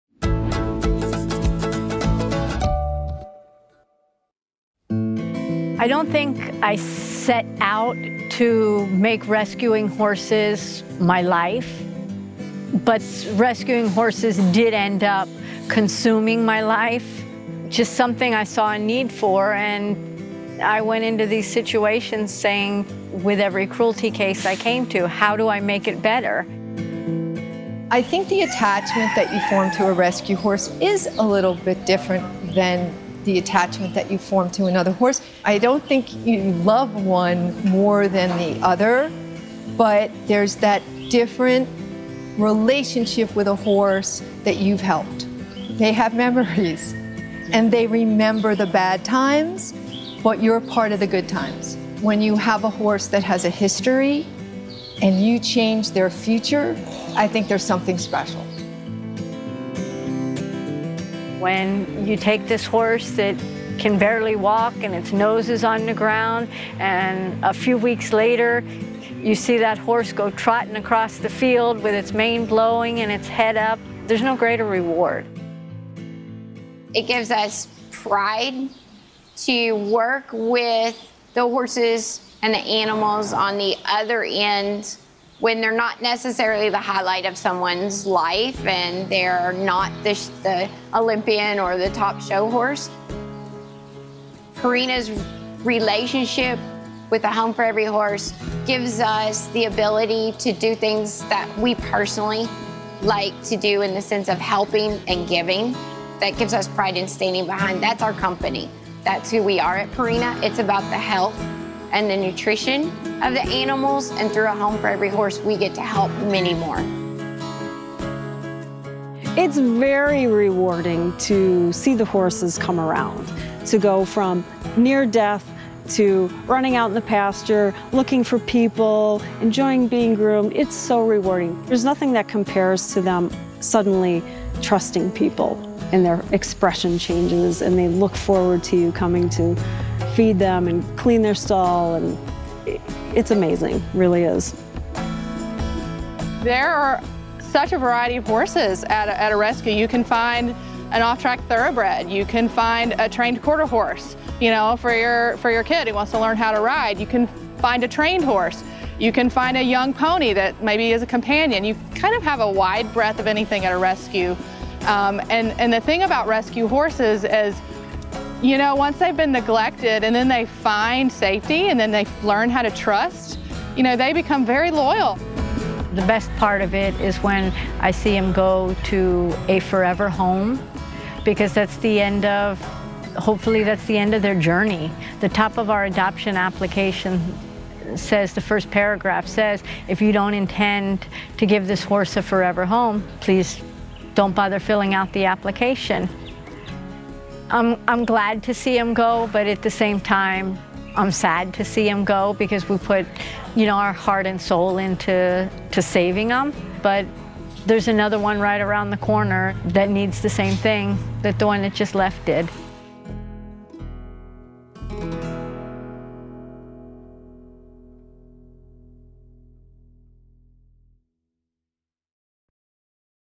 HFEH Miami SPCA Rescue Horses
Rescues horses give you a different relationship, they have a history that they will never forget. The Florida SPCA makes sure that any adopter will give these horses a forever home and that their future is full of everything they need.